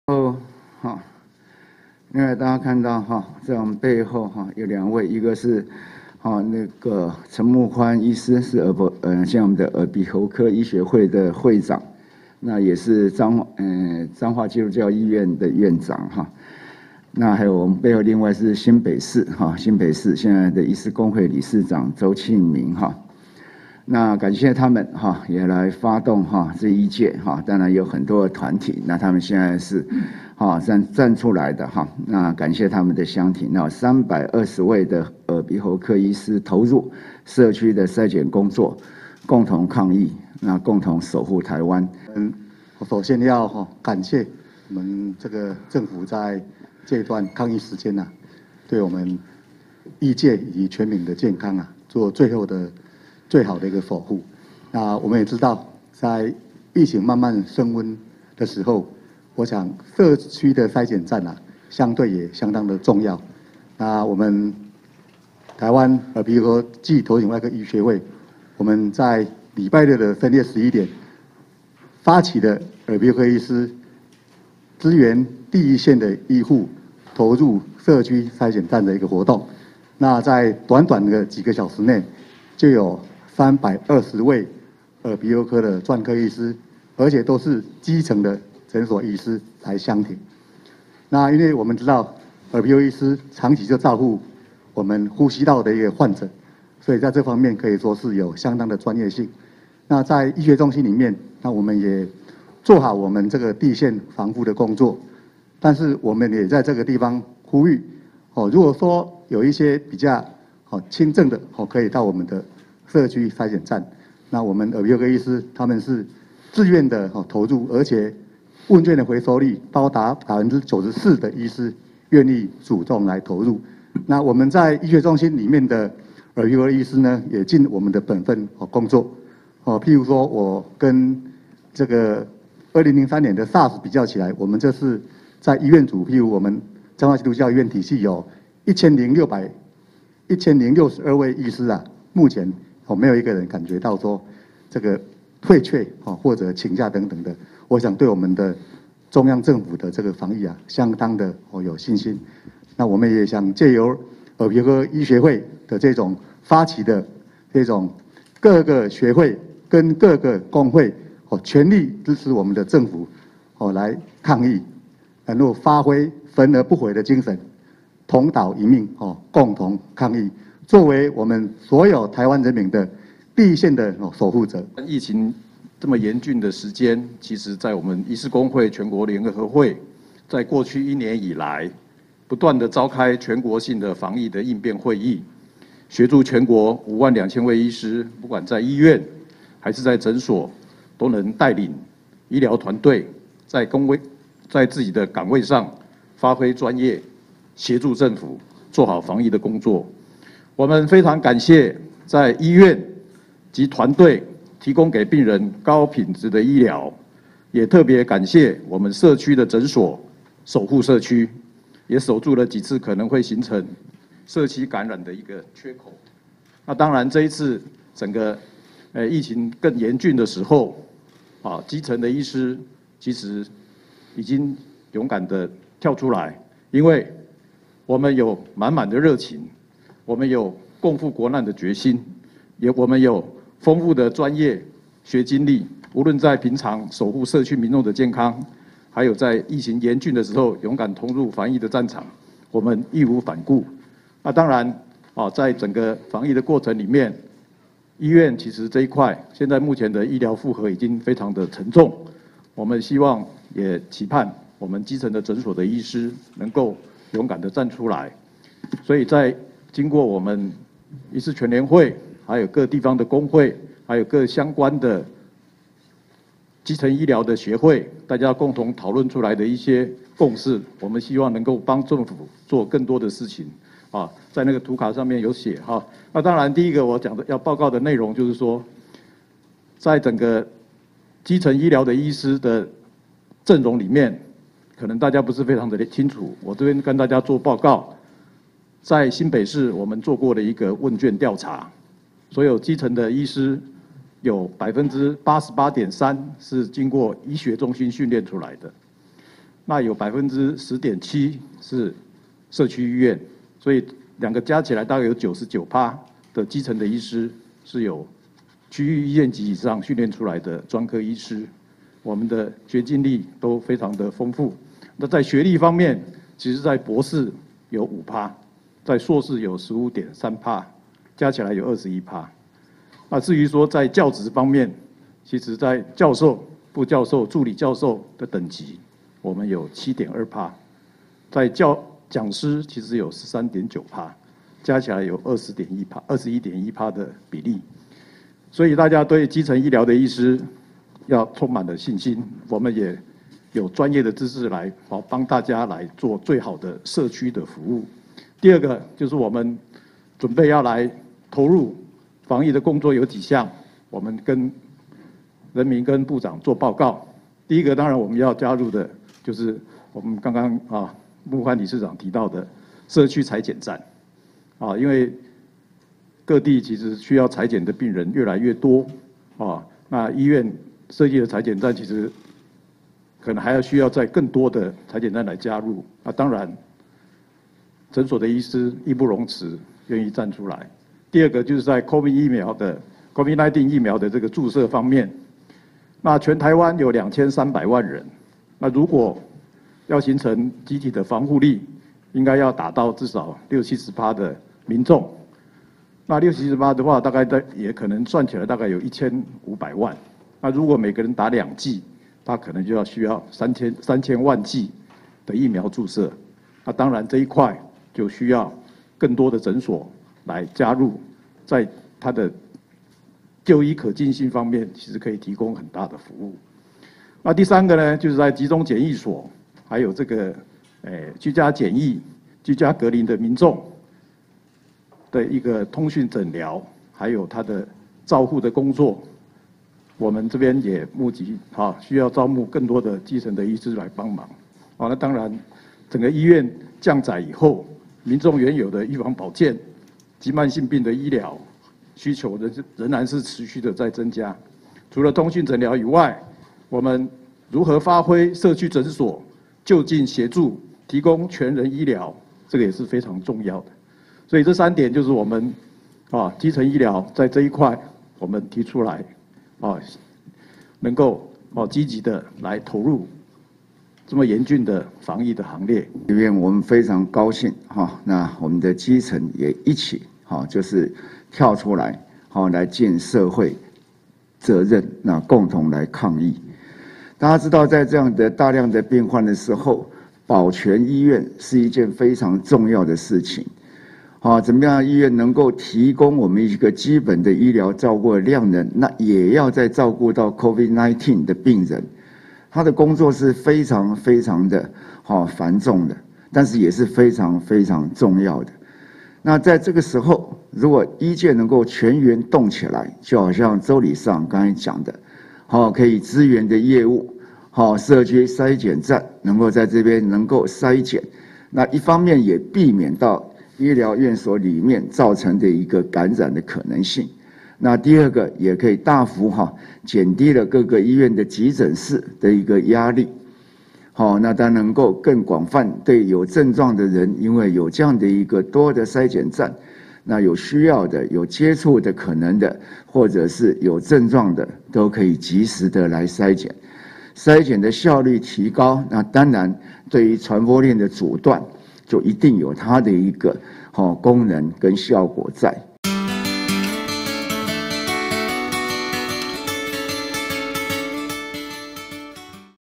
耳鼻喉科醫師相挺抗疫 投入社區篩檢站工作
耳鼻喉科醫師相挺抗疫 投入社區篩檢站工作 〔記者林明佑／彰化市報導〕為感謝台灣耳鼻喉頭頸外科醫學會號召全臺會員，投入政府成立的社區篩檢站，貢獻專業，共同抗疫，目前已有320位醫師願意參與。中央流行疫情指揮中心昨(18)日特別邀請台灣耳鼻喉頭頸外科醫學會理事長陳穆寬教授及新北市醫師公會理事長周慶明醫師蒞臨「嚴重特殊傳染性肺炎記者會」記者會，鼓勵醫界一起響應，共同抗疫。@changhuanews